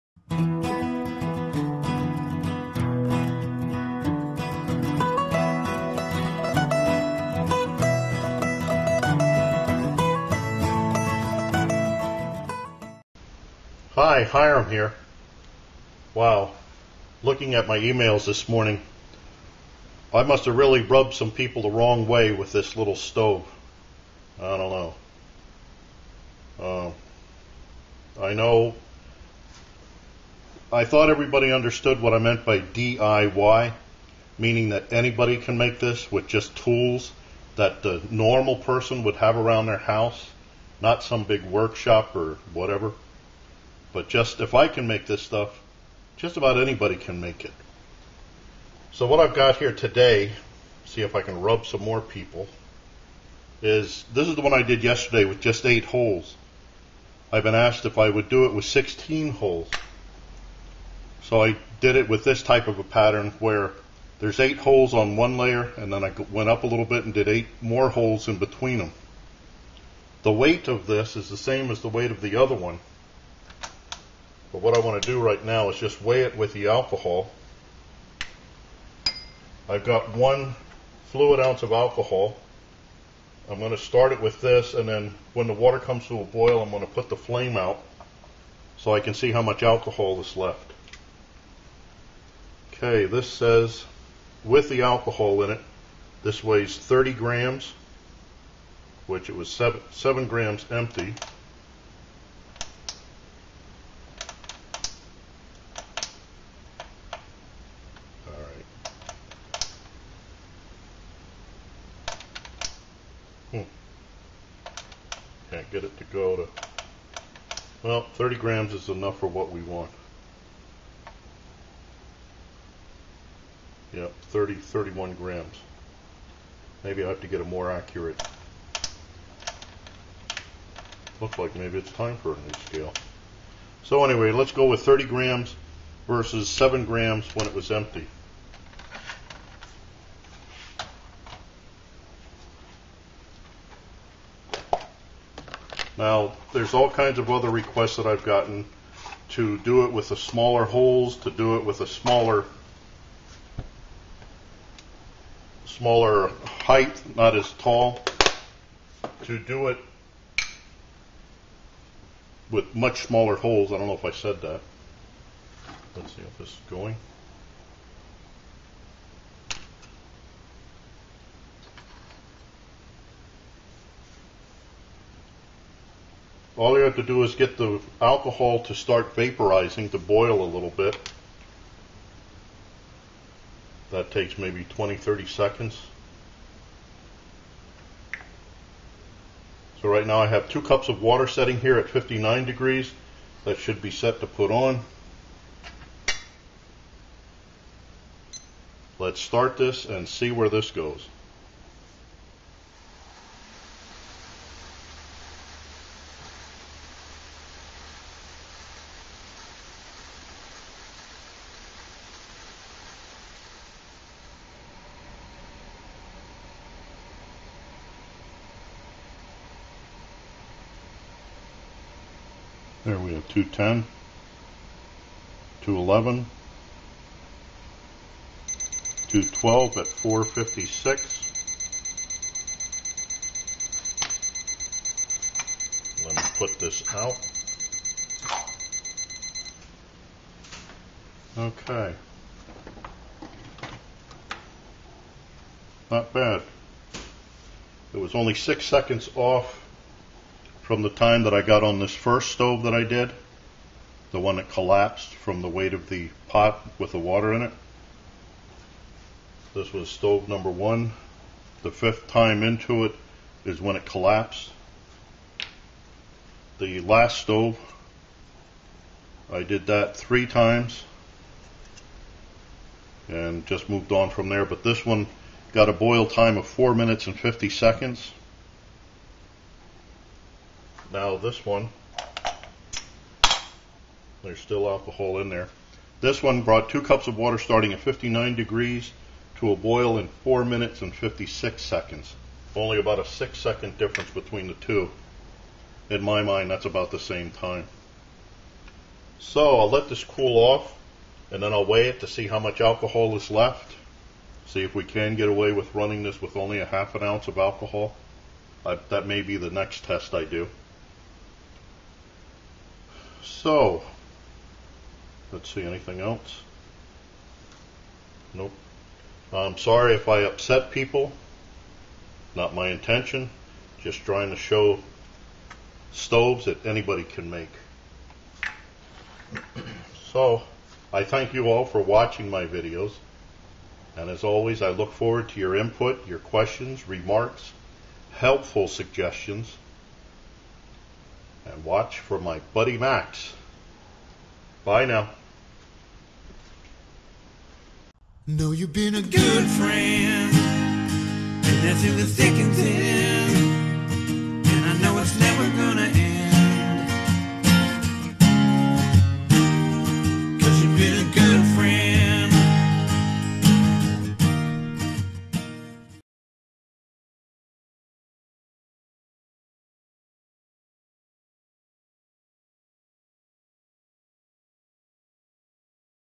Little Soda Can Stove V2.1 Boil Test 1
In this video I show another version of the Little Soda Can Stove (LSCS) where I've gone to the sixteen 3/16" holes on two levels. I have several more suggestions to be tried out.

This test was don't after I had already burned the stove four times with the same amout of alcohol and water. That way the tests shows how well the stove holds up.

A side note:  Looking at the emails I got this morning it seems that I've rubbed a couple of people the wrong way with this series of stoves. The point I'm trying to make, but seems to be missed, is that light-weight stoves can be made without specialized tools. If I can make these stoves just about anyone can.

Lab0192
Title: Little Soda Can Stove V2.1 Boil Test 1
Room temp: 78°
Humidity: 73%
Weather condition: Cloudy - about to rain
Burner: Little Soda Can Stove (LSCS) V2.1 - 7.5 fluid ounce size can
Stove/Pot stand: Little Soda Can Stove (LSCS) V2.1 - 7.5 fluid ounce size can
Pot: Boy Scout Pot #1
Fuel: alcohol,  denatured
Amount of fuel: 30 ml (1 fluid ounce) - 23 grams to start - 7 grams after burn
Amount of water: 2 cup(s)
Water temp at start: 59°
Time to boil: 4 minute(s) 56 second(s)
Time to run-out: NA
Water temp at run-out: NA
Note: